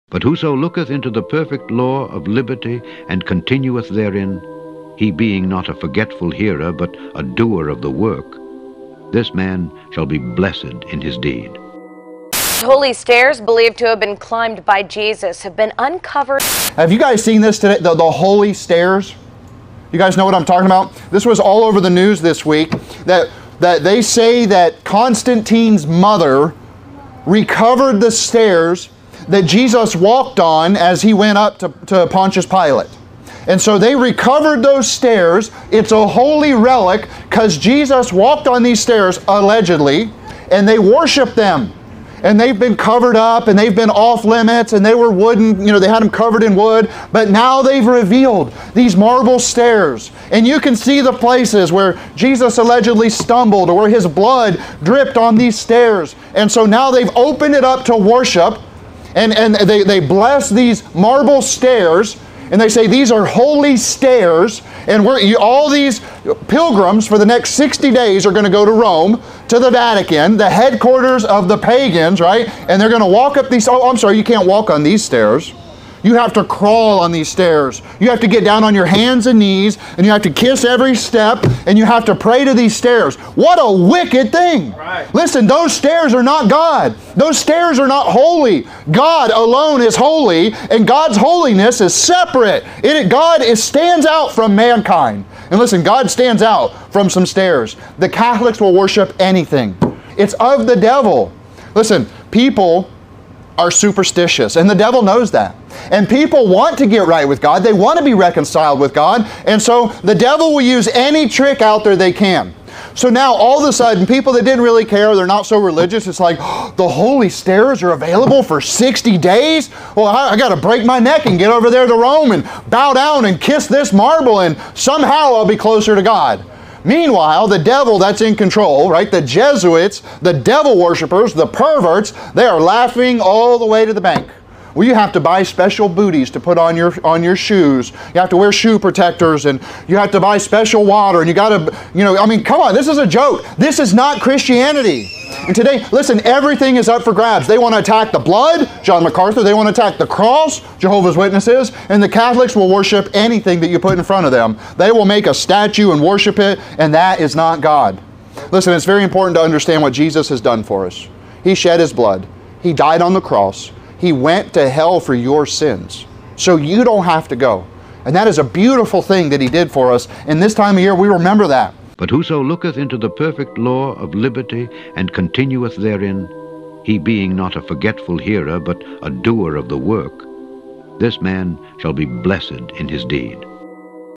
Holy Stairs! Vatican's New Idol
The Scala Sancta are a set of 28 white marble steps that are Roman Catholic relics